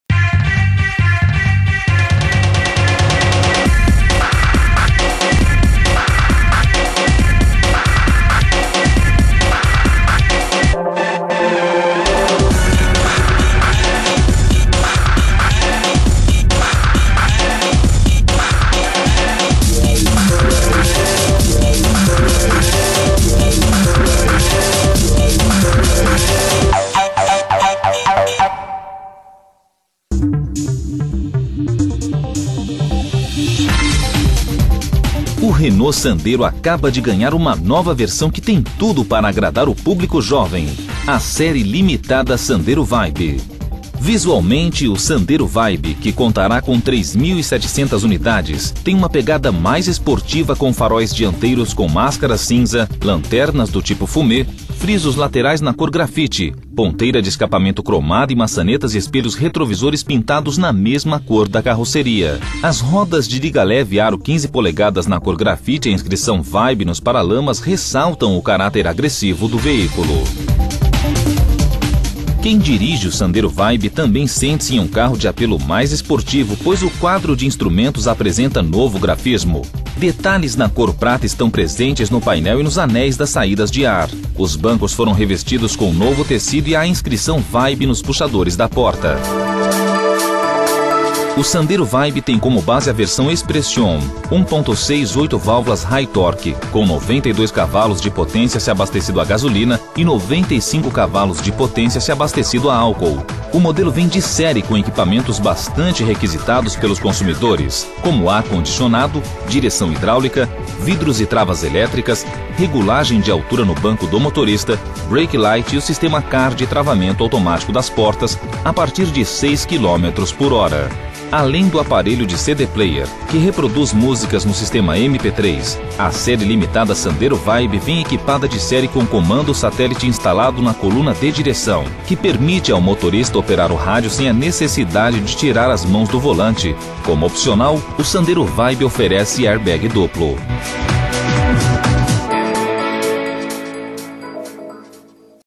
Renault Sandero Vibe
Renault lança série especial Sandero Vibe - Combinar uma ampla oferta de equipamentos de série a um visual esportivo, que agrade, principalmente, ao público jovem, esta é a receita da nova série limitada da Renault do Brasil, o Sandero Vibe. Esta nova série especial é limitada a 3.700 unidades e chega ao mercado com detalhes exclusivos, ampliando a gama do hatch que é sucesso de vendas no País.